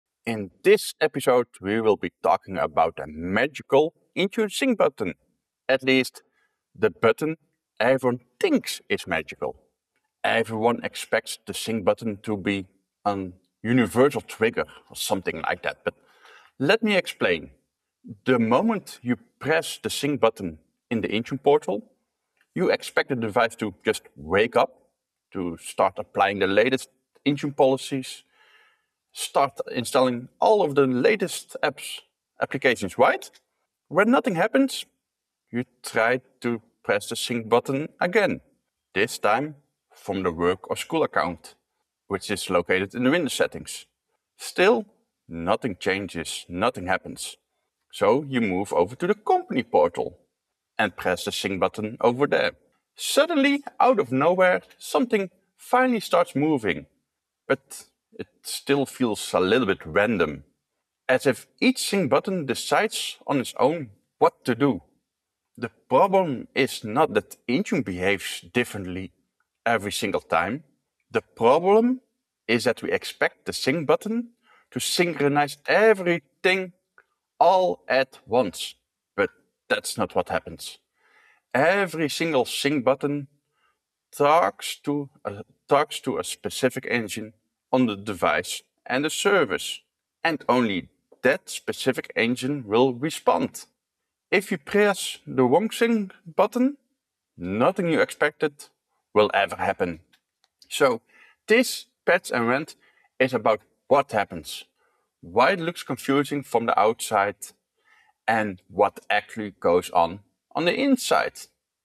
Intune Sync Button: The REAL Truth Revealed! | Patch and Rant Ep19
Unravel the mystery of the Intune sync button! Discover why it seems random and how to make it work for you. We explain the hidden engines behind the magic and clear up the confusion.